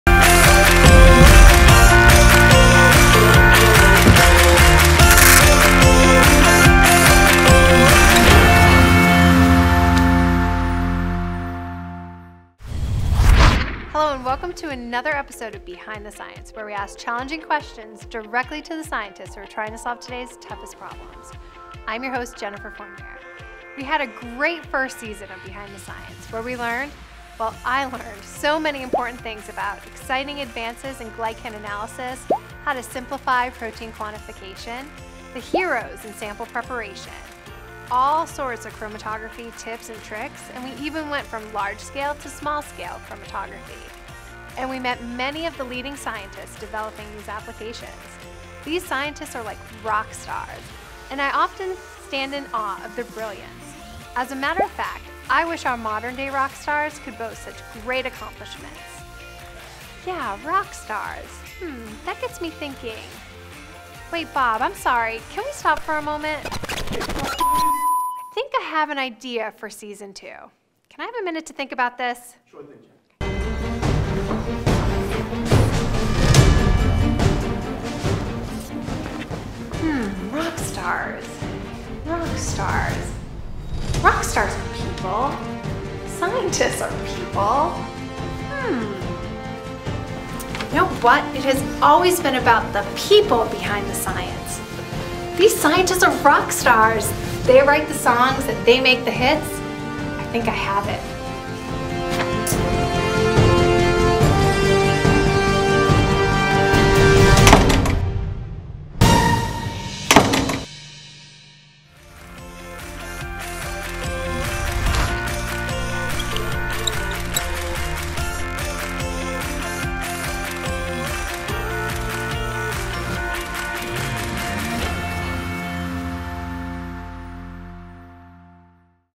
Behind the Science: A new idea!
Our host Jen Fournier looks back at the first season of Behind the Science: Advances in glycan analysis, how to simplify protein quantification, the heroes of sample preparation, and all the great scientists she's met across 22 episodes. That has Jen thinking. "These scientists are like rock stars, and I often stand in awe at their brilliance. As a matter of fact, I wish our modern-day rock stars could boast such great accomplishments..." And an idea for Behind the Science's second season starts to blossom!